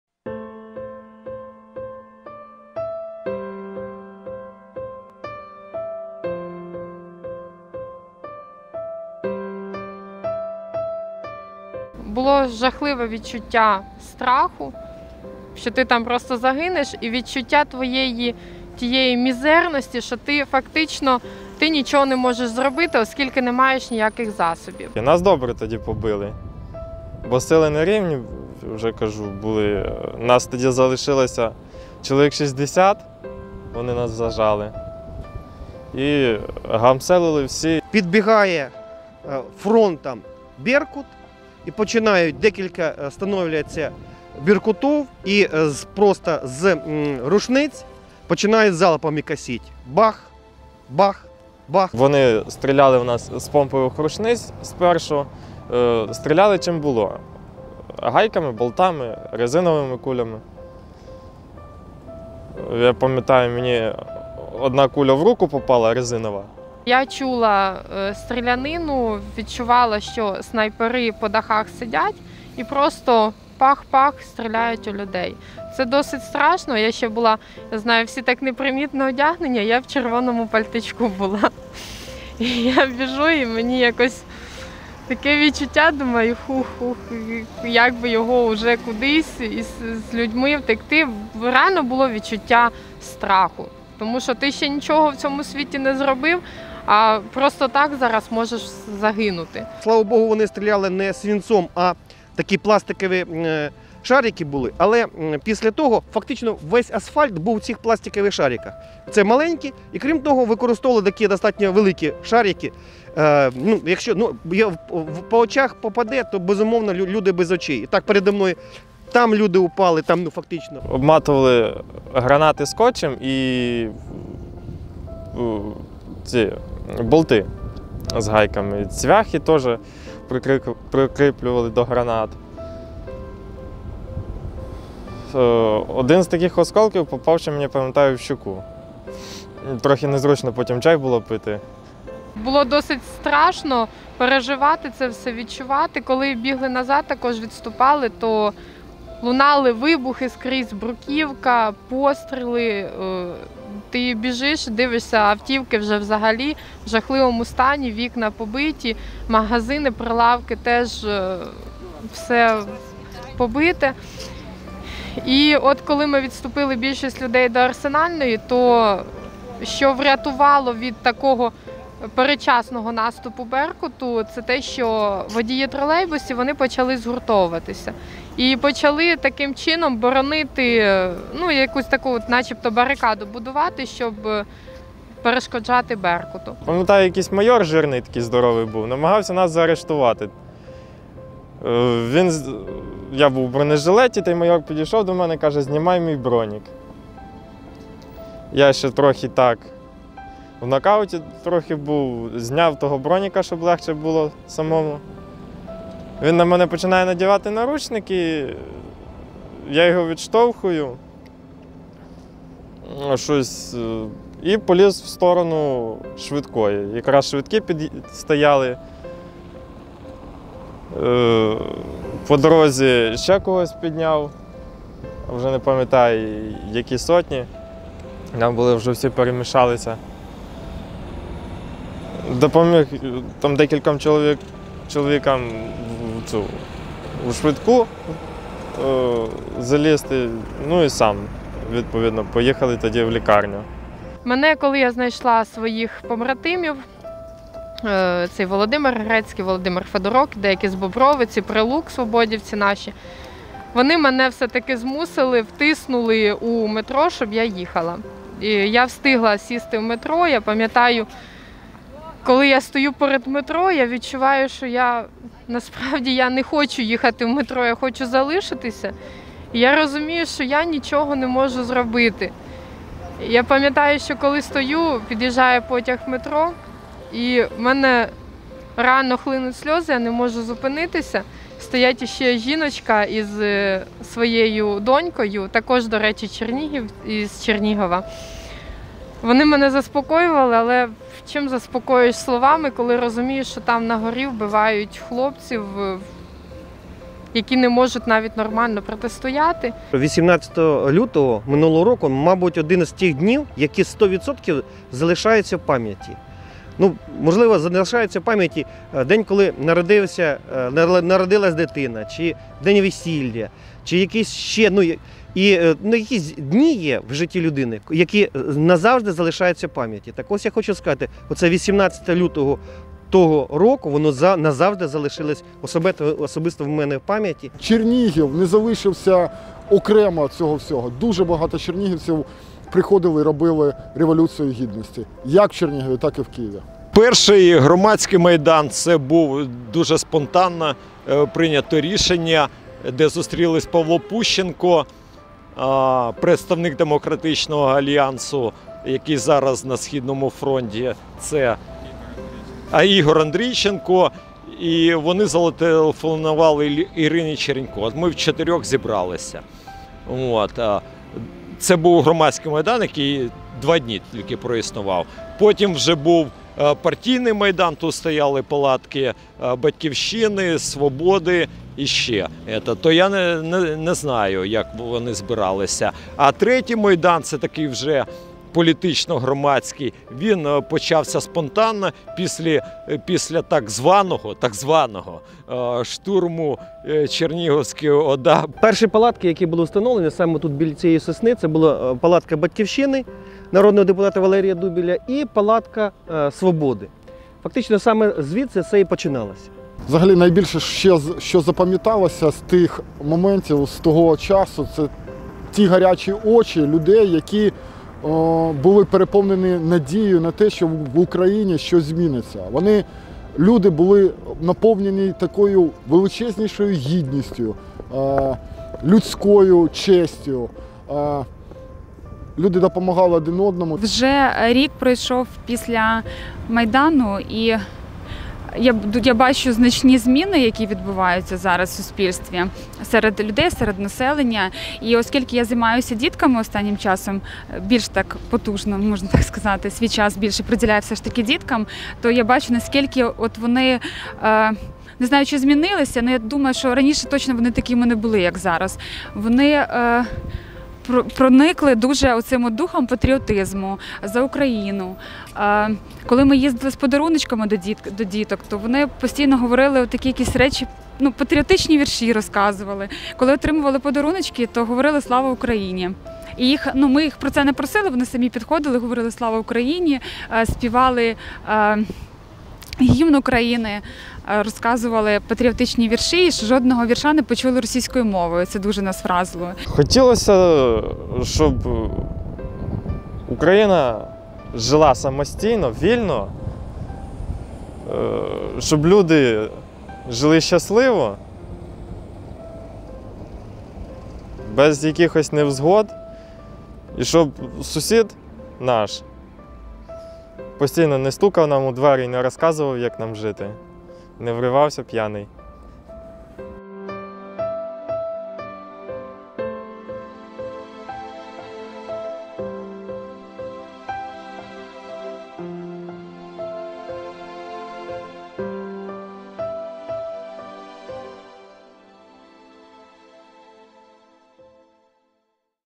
Чернігівські очевидці розповіли про події у Києві 18.02.14
Garmata TV